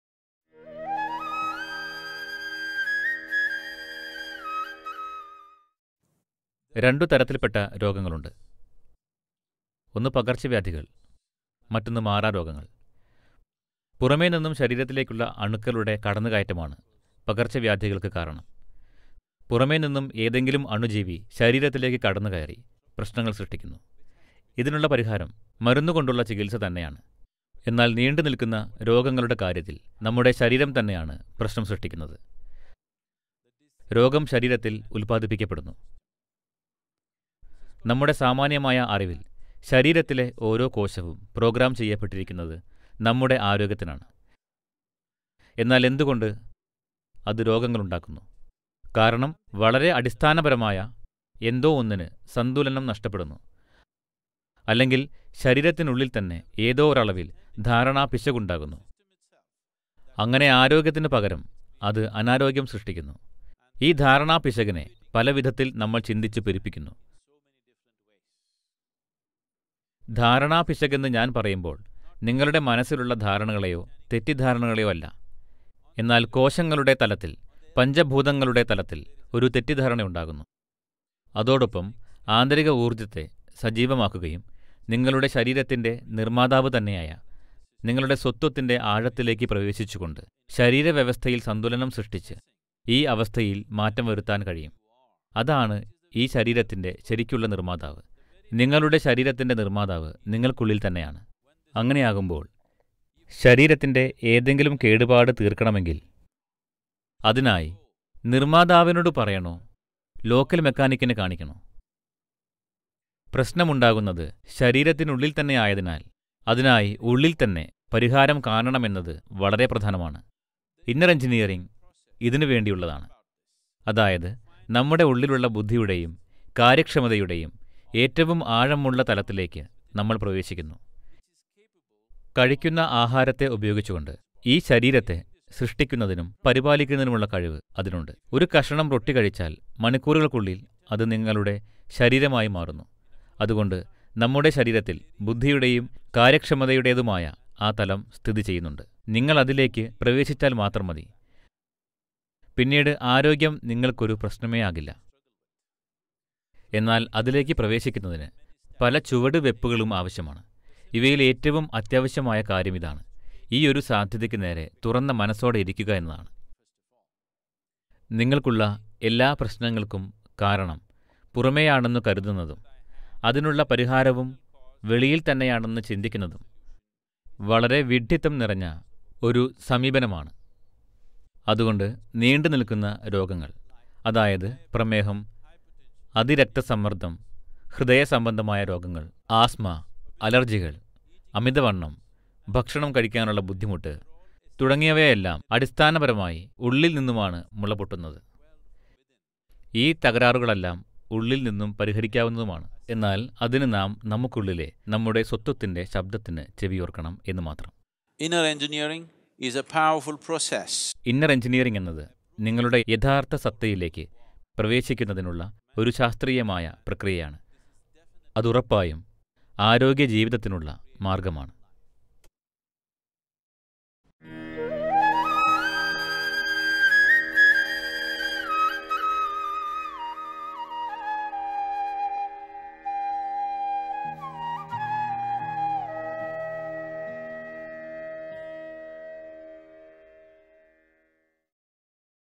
പ്രമേഹവും രക്തസമ്മർദ്ദവും എങ്ങനെ ഇല്ലാതാക്കാം ? Chronic ailments Hypertension, Diabetes , Asthma
നമ്മുടെ സ്വന്തം ശരീരത്തിനകത്തെ പ്രശ്നങ്ങൾ, എങ്ങനെയാണ് നീണ്ടു നിൽക്കുന്ന രോഗങ്ങൾക്ക് കാരണമാകുന്നത് എന്ന് സദ്ഗുരു  വിശദീകരിക്കുന്നു. പ്രമേഹം, രക്ത സമ്മർദ്ദം തുടങ്ങിയ നീണ്ടു നിൽക്കുന്ന  രോഗങ്ങൾ, ശരീരവ്യവസ്ഥക്കുള്ളിലെ ചില അസന്തുലിതാവസ്ഥയെ ആണ് സൂചിപ്പിക്കുന്നത്.